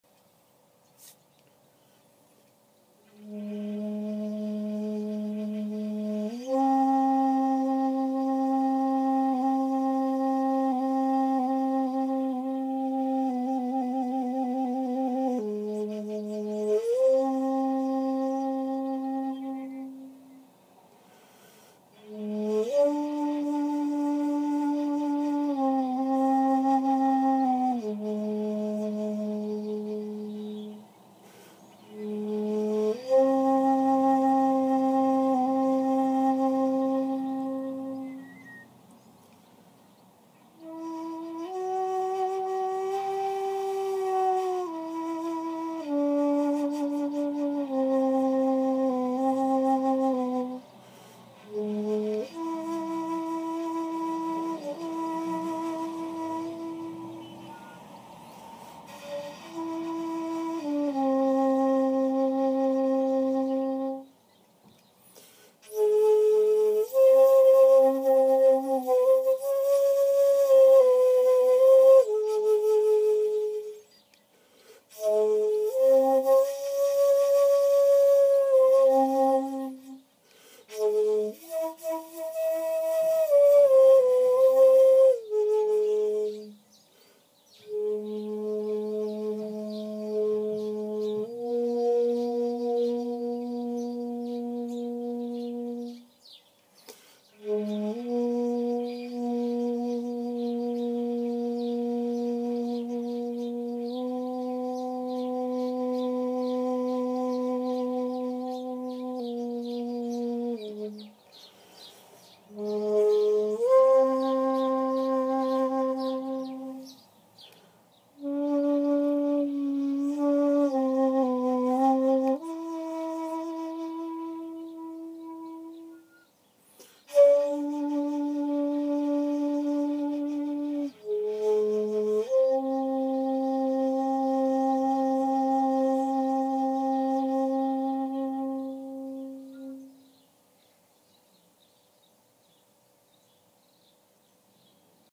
Ney Shaku
This is a bamboo ney with shakuhachi style mouth piece, can go up to 3 octaves.